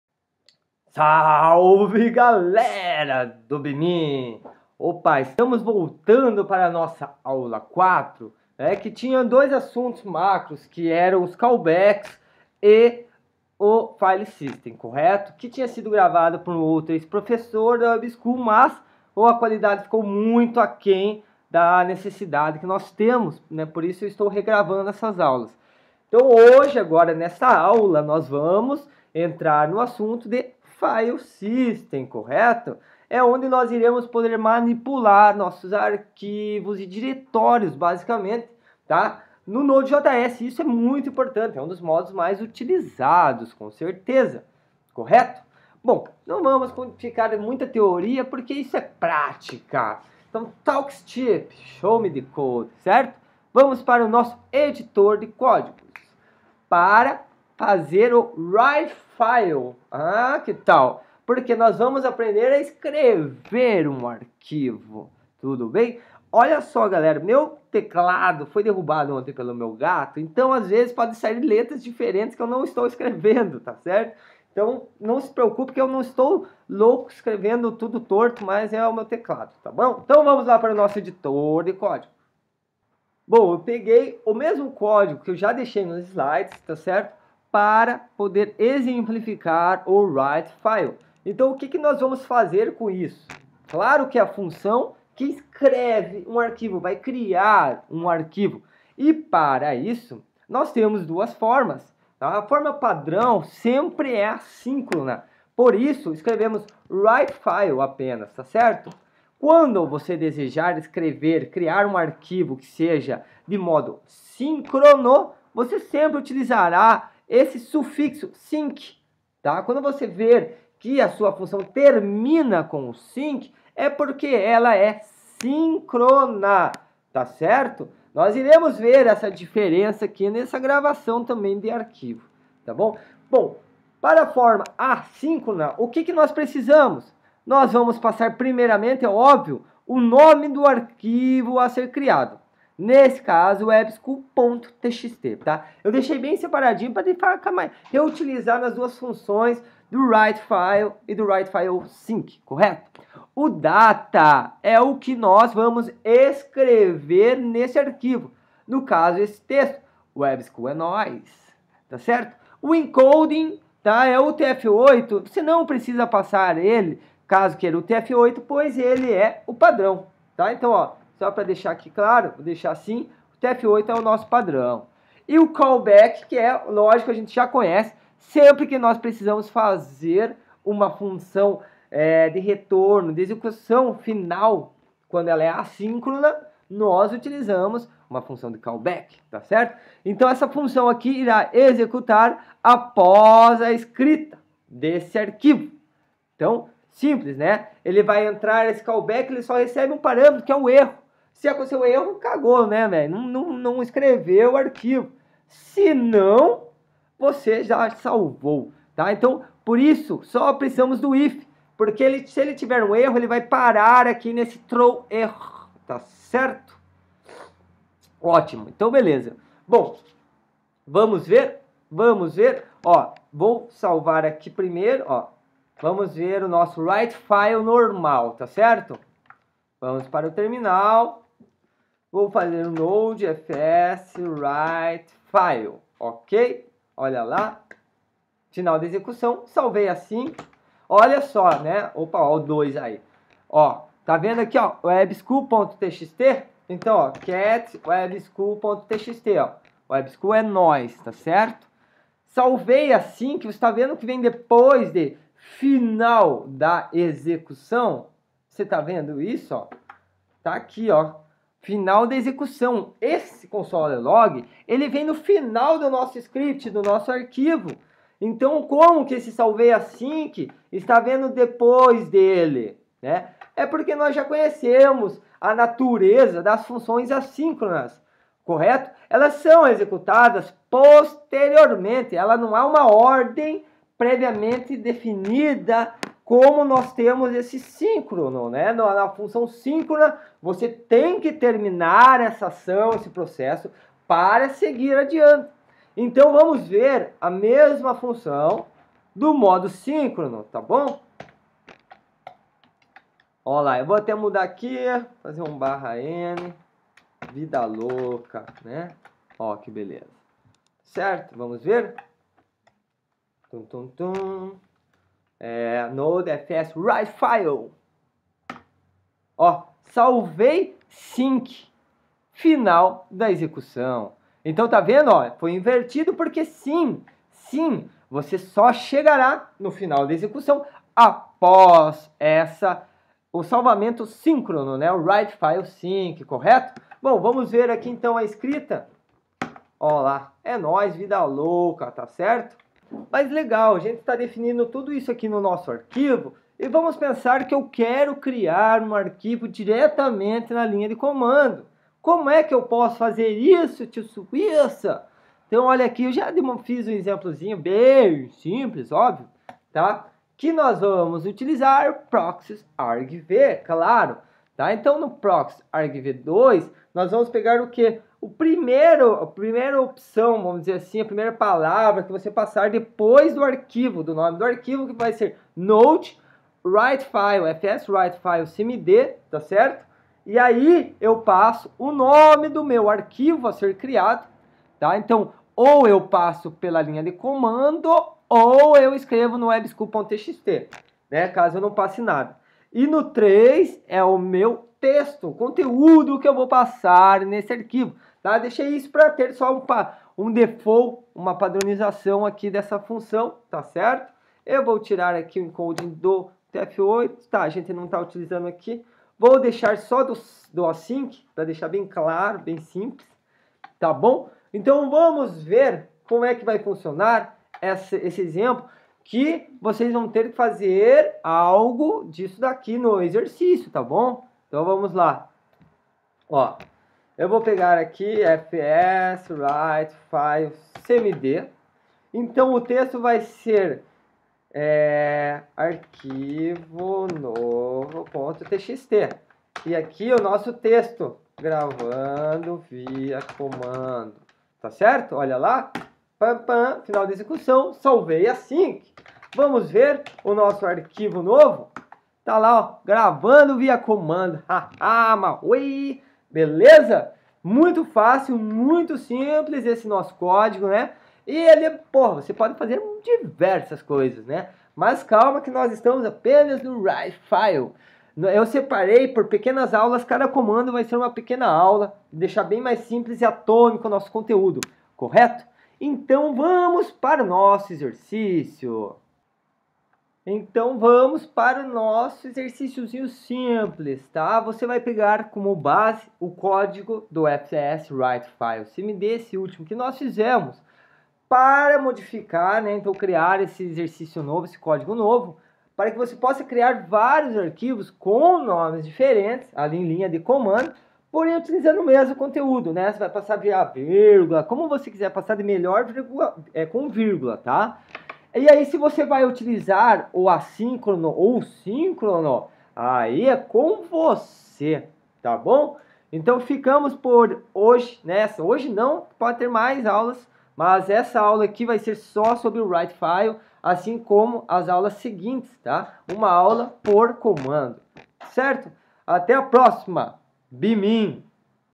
Be MEAN: Node.JS (AULA 4) - 02 FileSystem - Criando arquivos com writeFile
AULA REGRAVADA sobre FileSystem!

Início da série de aulas sobre o FileSystem com Node.js iniciando com o writeFile.